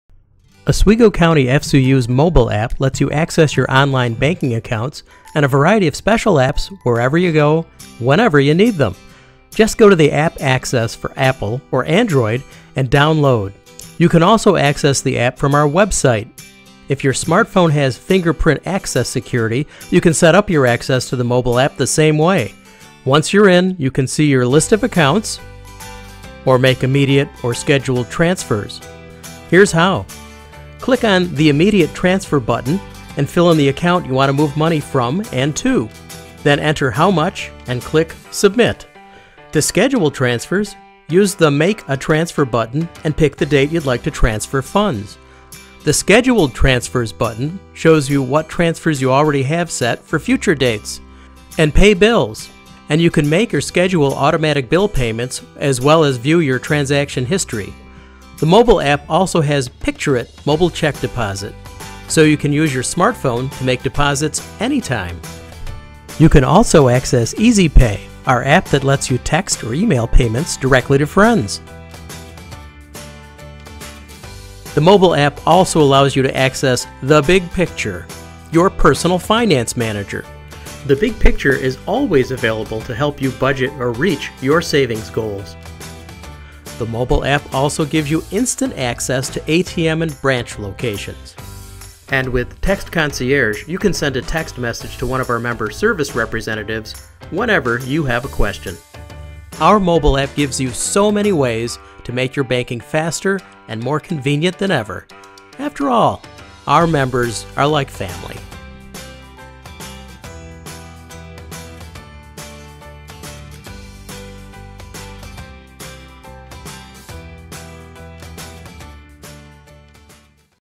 Mobile App Tutorial
Tutorial on all the features of Oswego County Federal Credit Union's Mobile App.